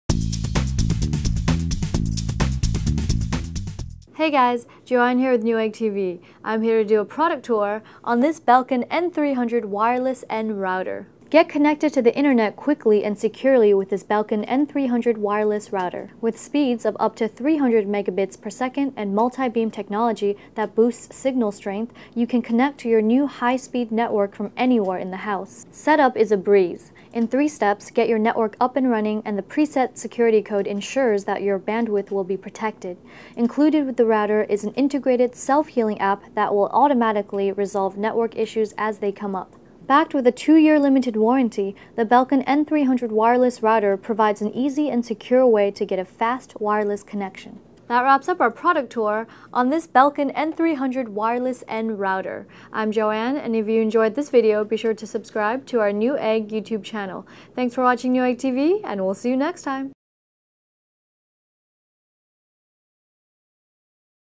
Newegg TV: BELKIN E9K3000 Wireless N300 Router Product Tour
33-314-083

Here's a Wireless N300 Adapter, made by BELKIN, Model: E9K3000. Check out the video for more info!

BELKIN E9K3000 Wireless N300 Router IEEE 802.3/3u, IEEE 802.11b/g/n

- Credits -
Presenters: Joanne
Producer: Lam
Script: Rachel
Camera: Anna
Post-Production: Anna